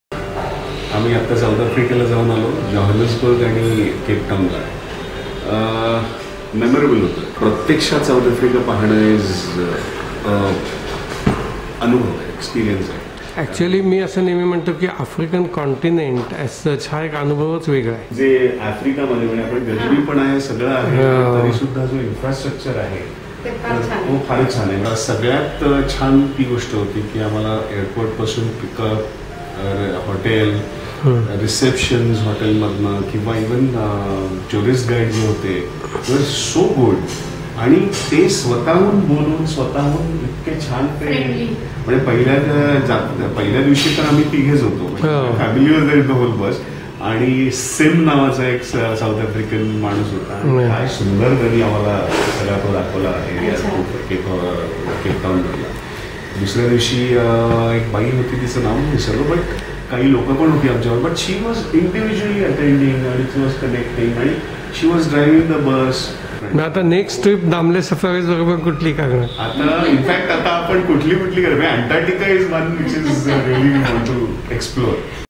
Sandeep Kulkarni - South Africa tour with Damle Safaris
Acclaimed Bollywood actor Sandeep Kulkarni recently spent vacation with family in South Africa. His tour was arranged by Damle Safaris. He gets candid in this interview with us.